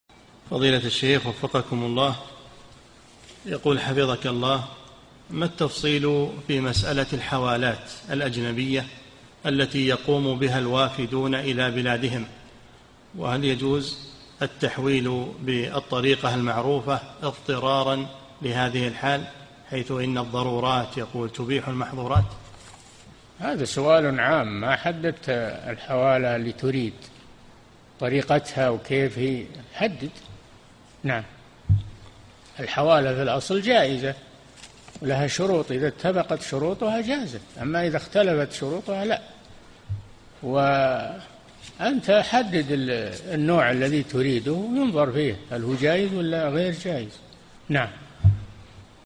[1753 -3022] هل يجوز تحويل الوافد المال إلى بلده بالطريقة المعروفة؟ - الشيخ صالح الفوزان
-"[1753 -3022] السؤال: حفظك الله، ما التفصيل في مسألة الحوالات الأجنبية التي يقوم بها الوافدون إلى بلادهم وهل يجوز التحويل بالطريقة المعروفة اضطراراً لهذه الحال؟ حيث إن الضرورات يقول: تبيح المحظورات؟
الجَوَابُ: هذا سؤالٌ عام ما حددت الحوالة التي تريد، طريقتها وكيف هي، حدد. الحوالة في أصل جائزة، ولها شروط إذا اتفقت شروطها جازت، أما إذا اختلفت شروطها لا، وأنت حدد النوع الذي تريده وينظر فيه، هل هو جائز أو غير جائز؟ نعم.
للاشتراك في قناة الشيخ صالح الفوزان

للاستفسار والإقتراحات والدعم
رئيس الفريق
لمشروع كبار العلماء